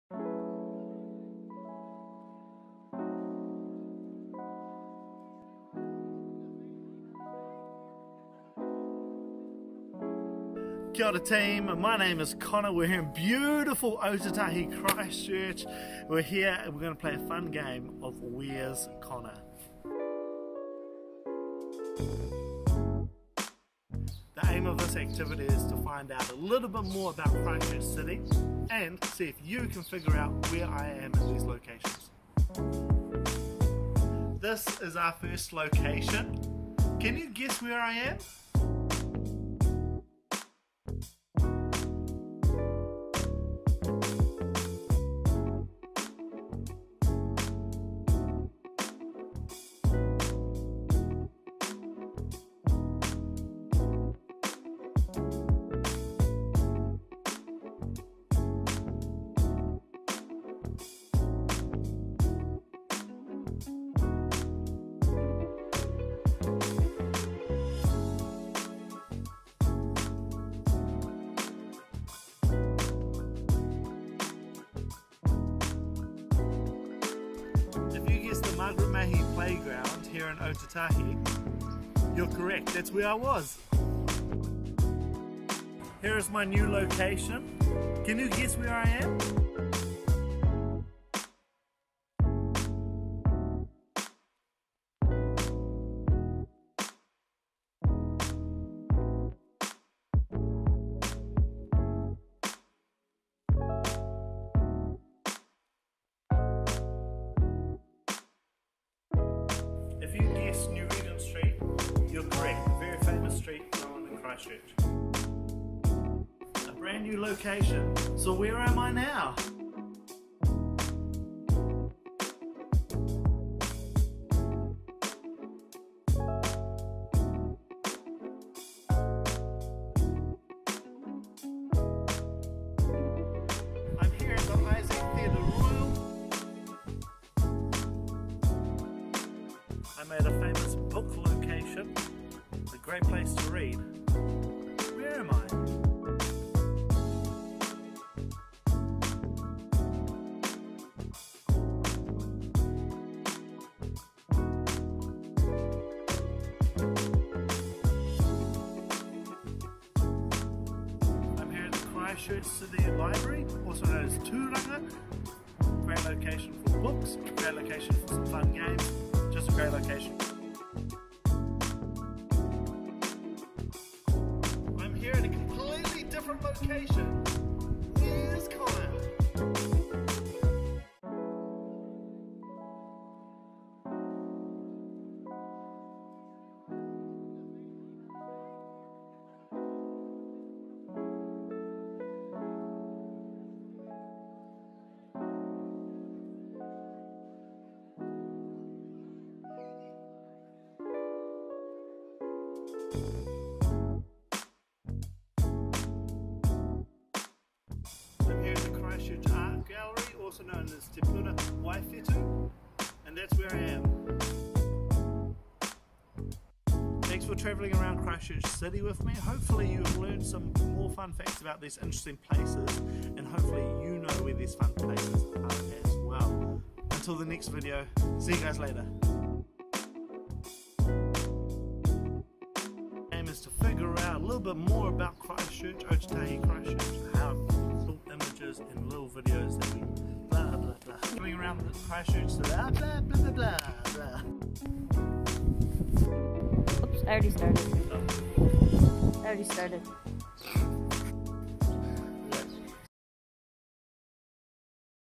Where is Connor?
Connor has visited some places in Christchurch, can you tell us where he is...?